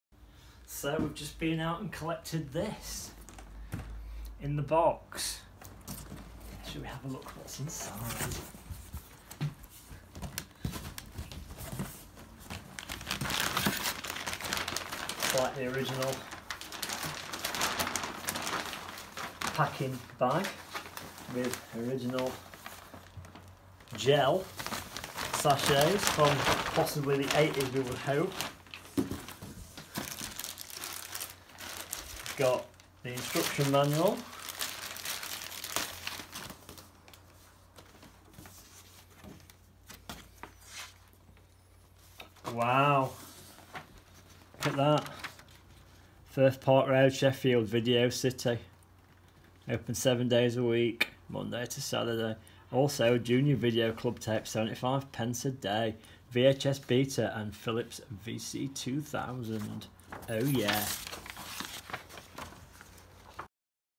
Unboxing Philips Video 2000 part one
Picked up a video 2000 machine with tapes for £25. In the original box with all instructions!